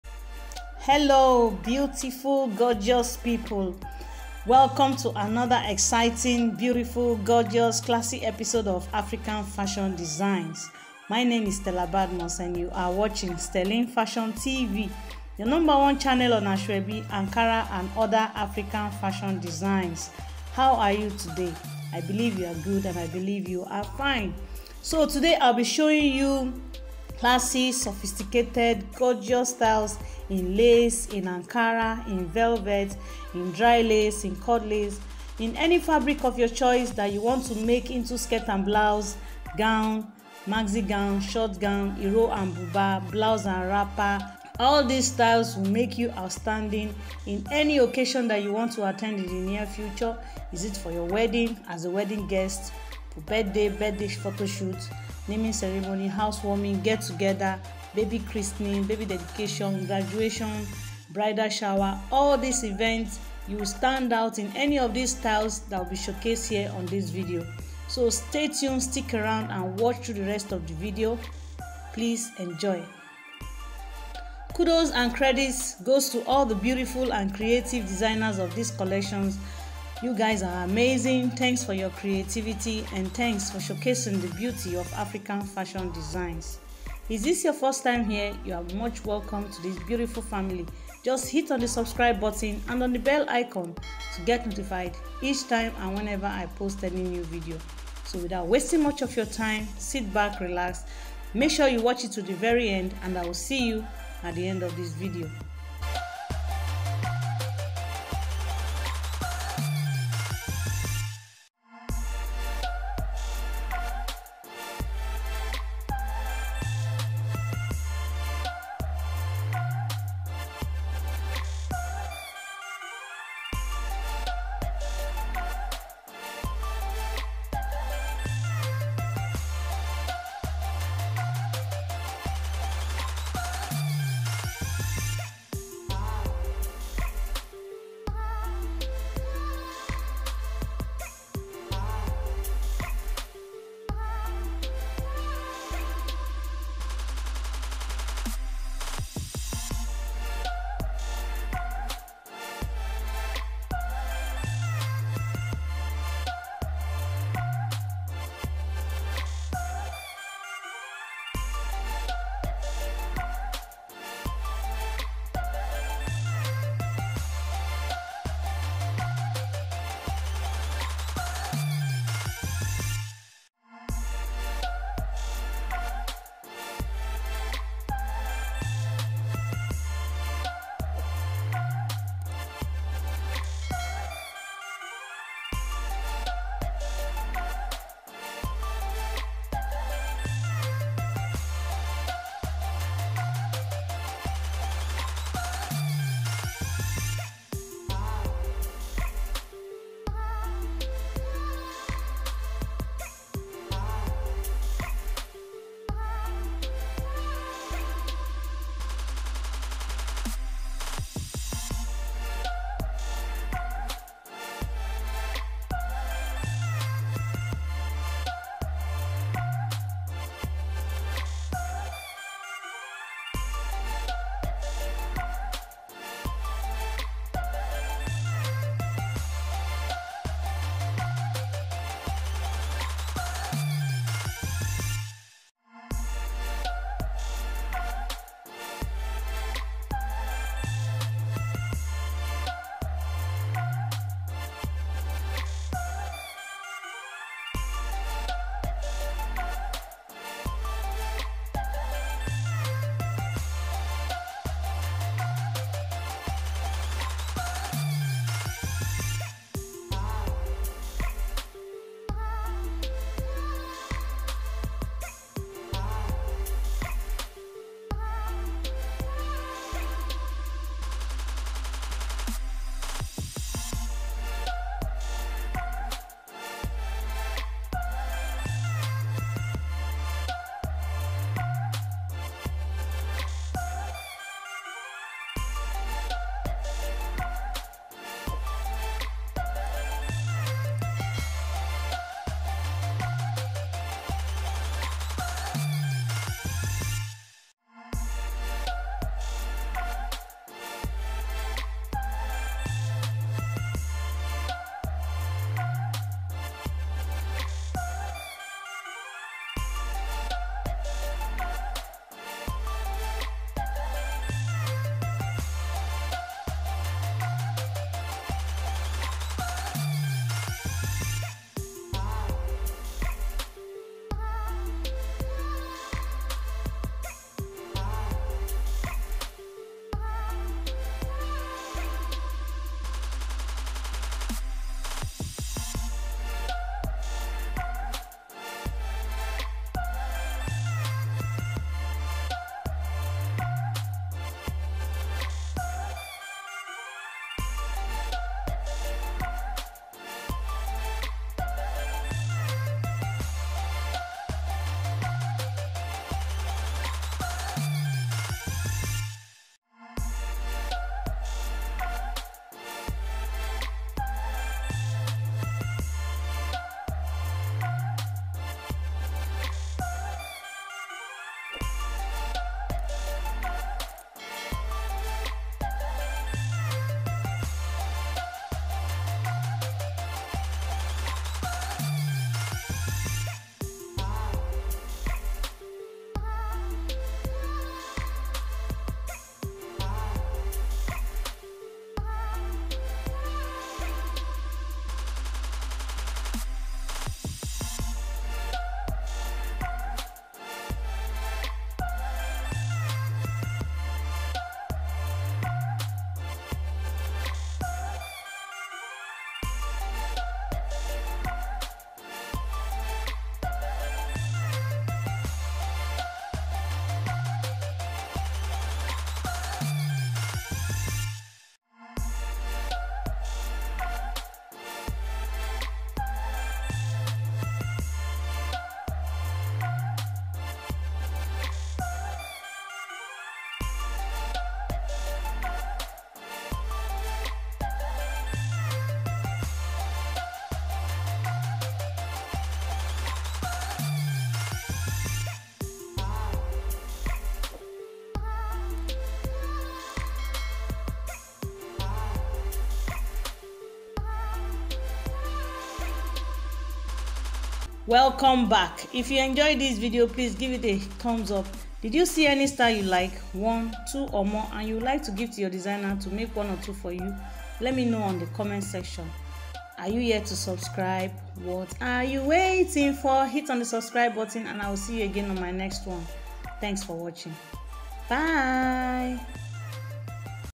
Ankara Styles 2022 | Most Latest Lace, Velvet & Ankara Styles For Owambe | Classy Asoebi Style 2022
Hello Fabulous, today you will be watching Asoebi/Owambe Styles you won't want to miss, so sit back and enjoy watching to the very end and let me know on the comment section what you think.